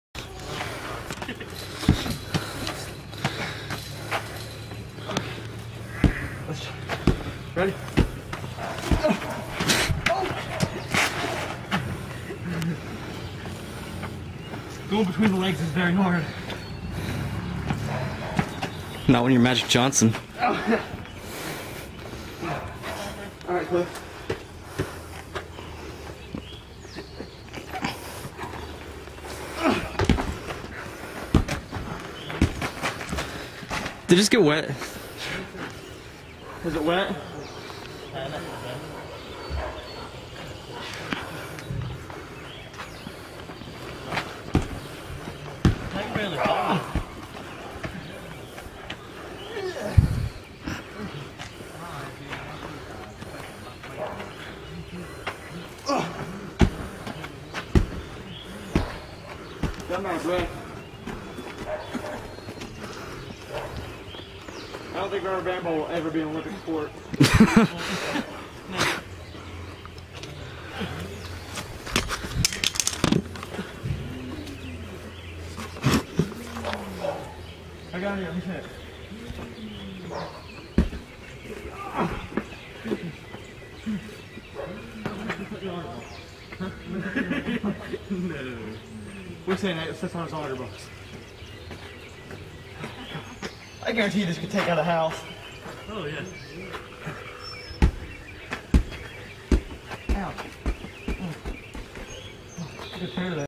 Bouncing a 50 lb rubber band ball
Solid rubber bands, straight to the core. The ball is now ~130 pounds!